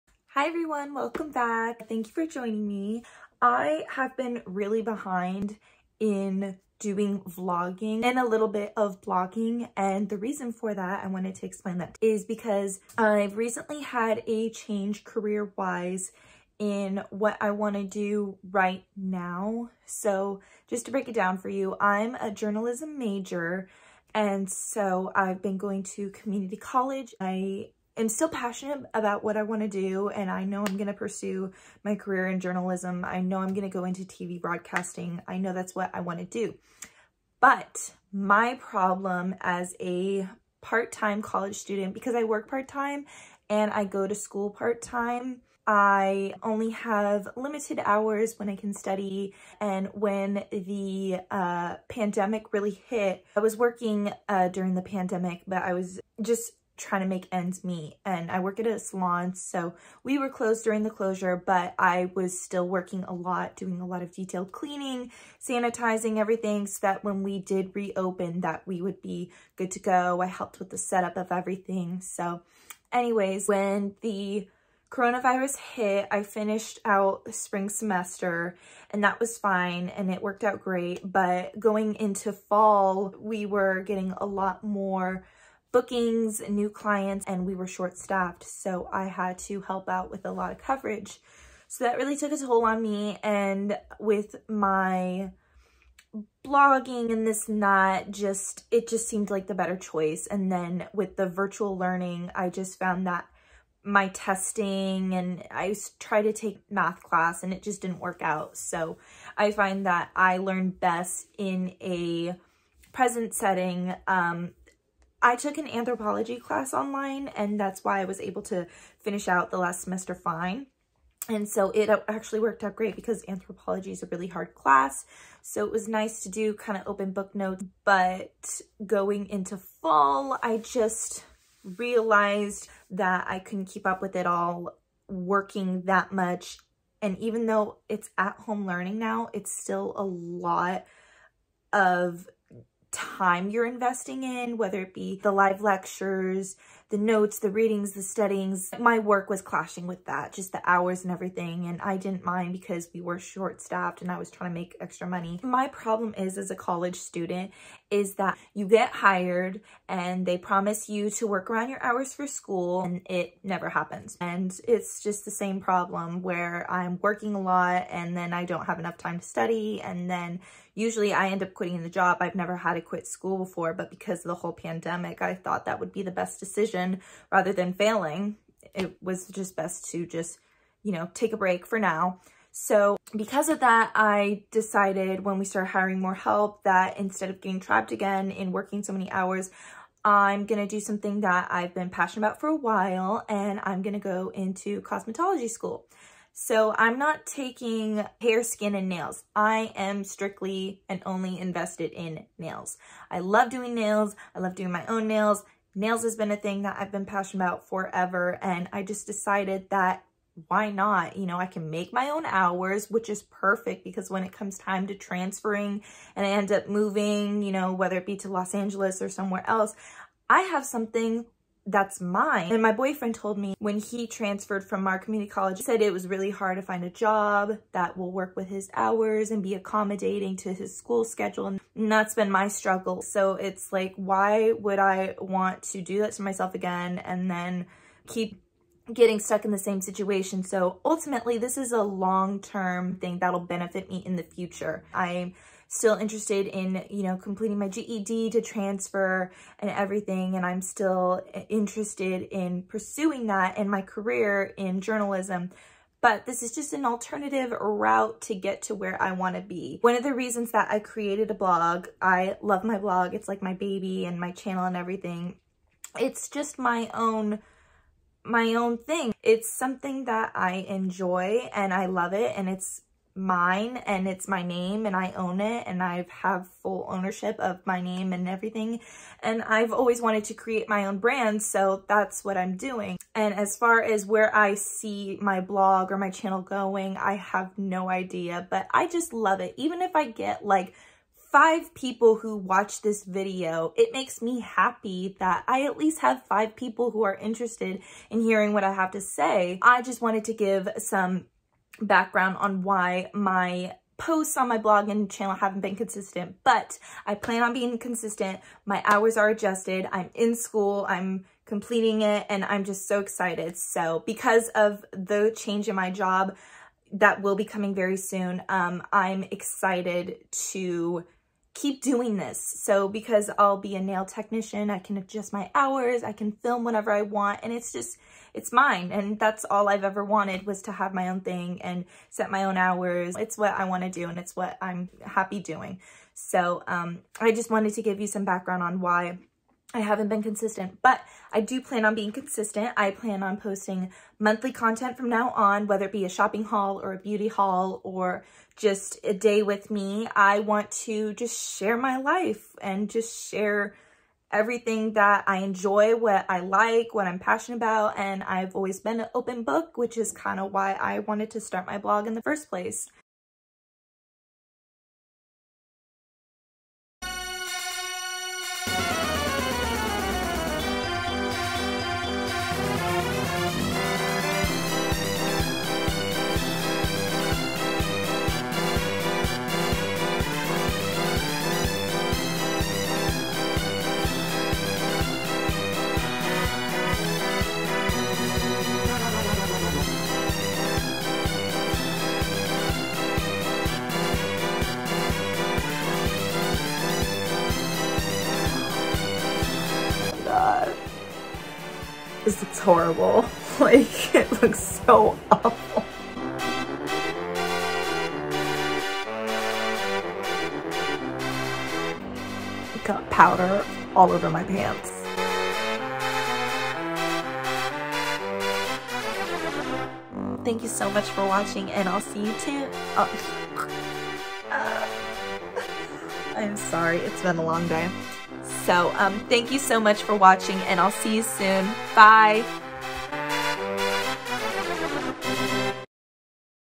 College struggles| bloopers
I get real about what I've been up to for the last few months, what working part-time as a college student is really like, and some bloopers.